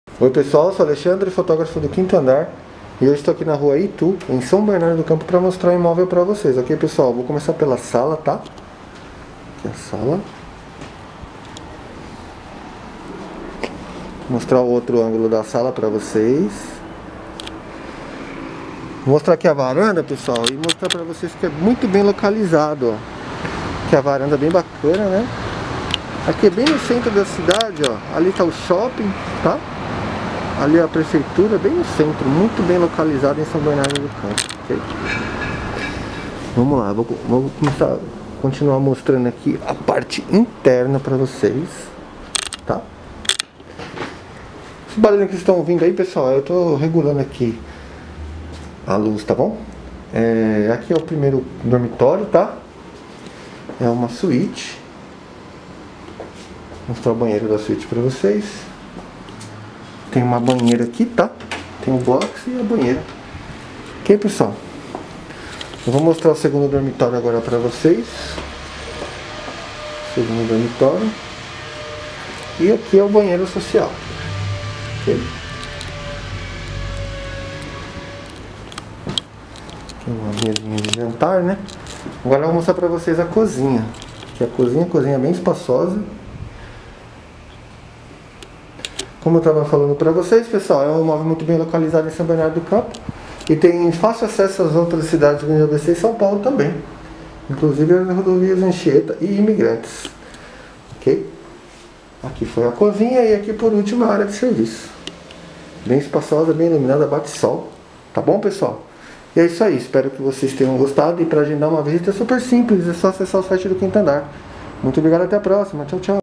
QuintoAndar Imóvel 893578668
Apartamento/Casa com 2 dormitórios, 134m².
Localizado na Rua Itu, Baeta Neves, São Bernardo do Campo.